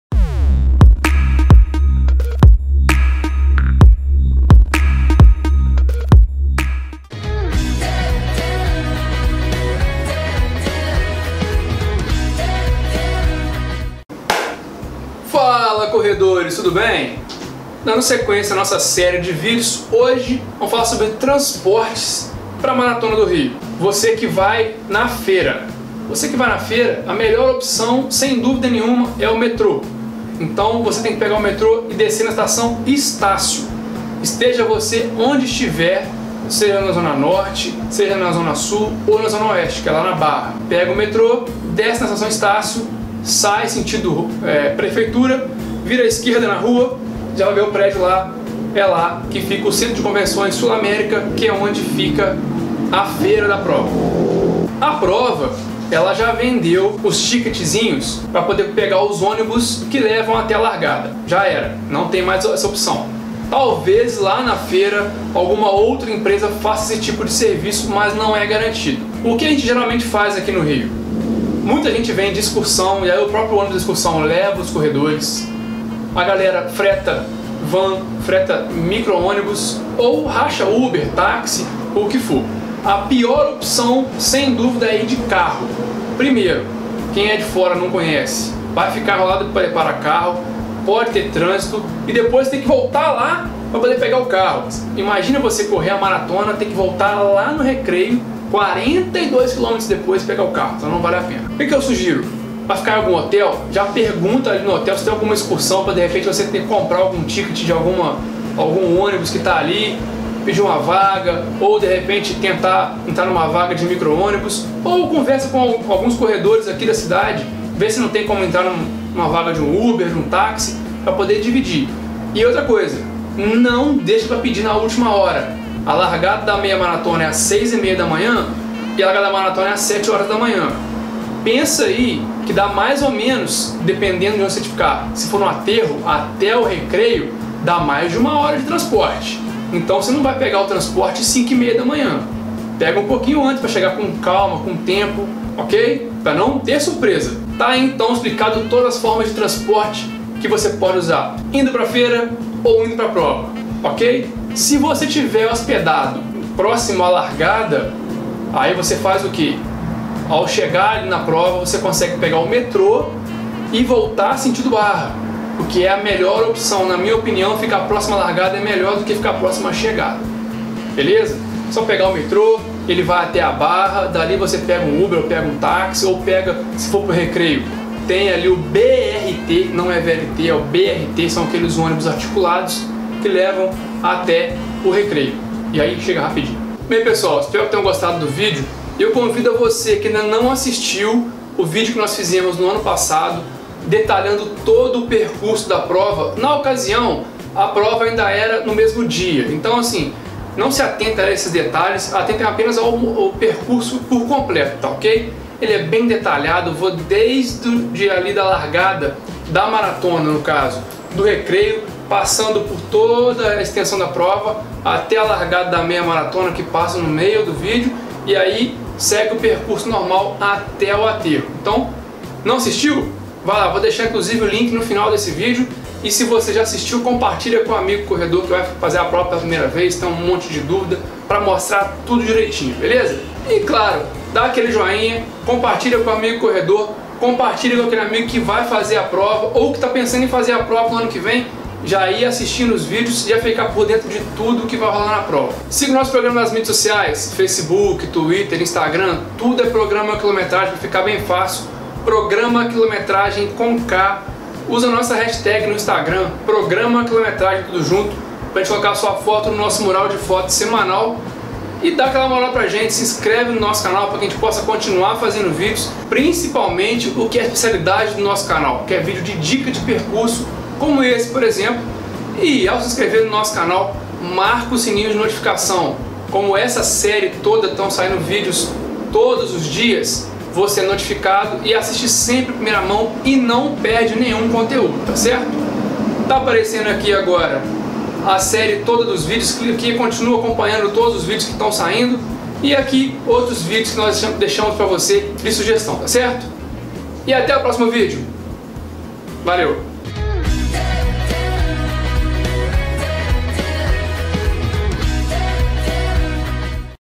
Especial Maratona do Rio 2018 #5 - Transportes para as provas
Faaaaaala corredores, tudo bem? Na nossa série especial da Maratona do Rio, hoje vamos falar dos transportes para as provas e para a feira. Então se liga nas dicas para saber como fazer!!